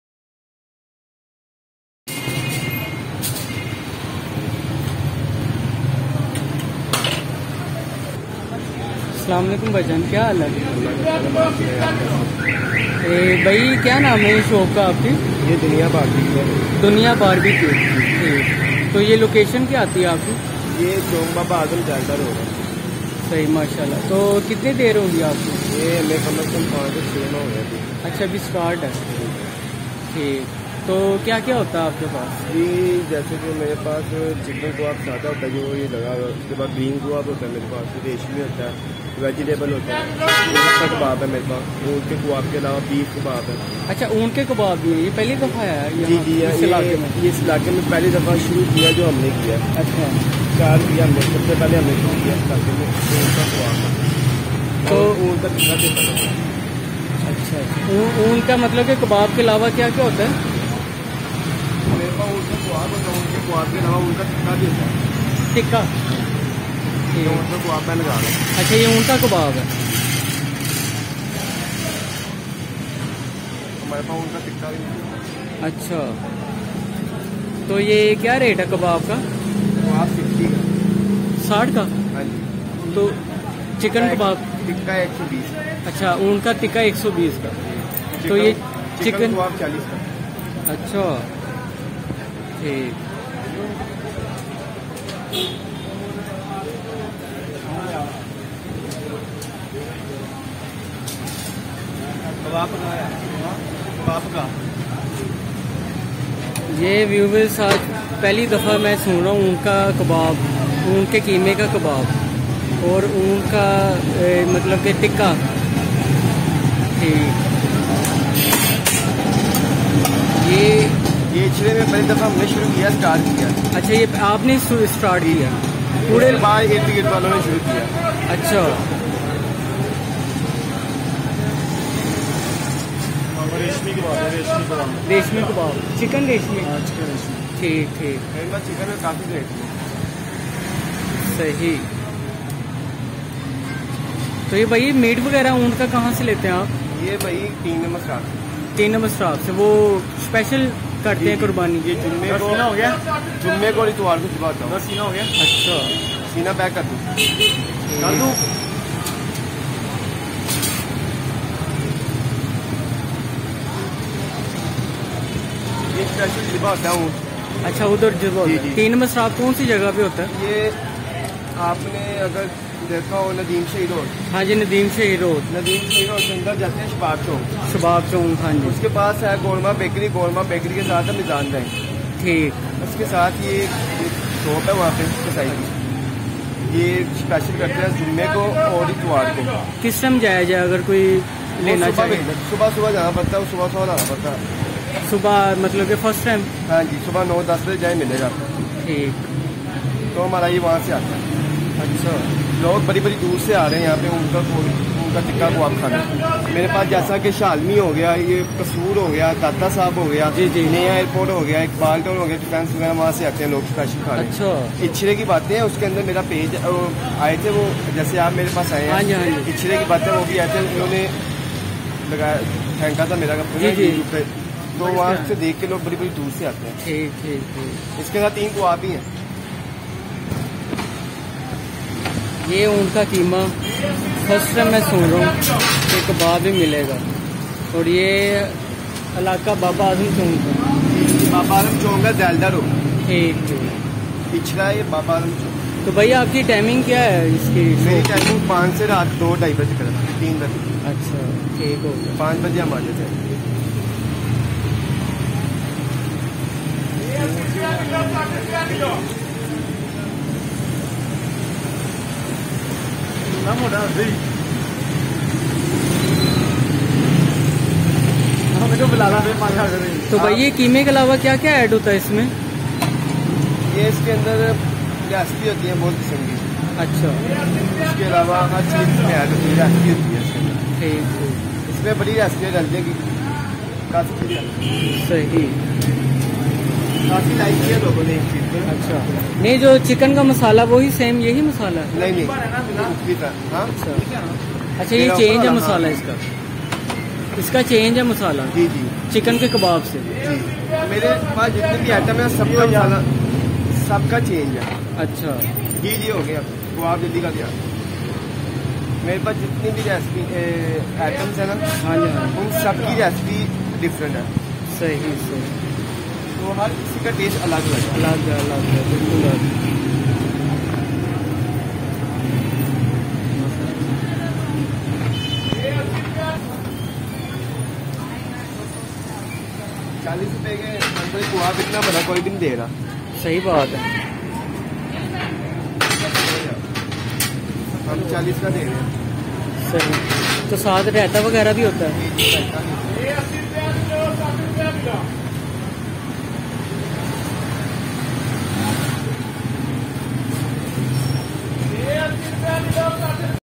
Camel Meat BBQ Kabab And Tikka, ( Street Food Ichra Lahore) Azad Foods
pahliy bar Camel k kabab or tikka
best camel meat and good shop
Location  baba azam chowk ichra lahore
Azad Foods#